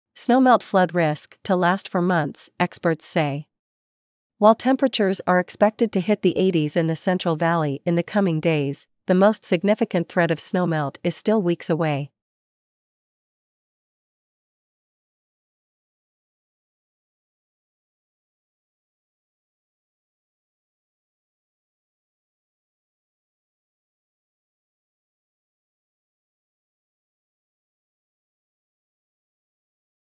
Snowmelt flood risk to last for months, experts say
While temperatures are expected to hit the 80s in the Central Valley in the coming days, the most significant threat of snowmelt is still weeks away.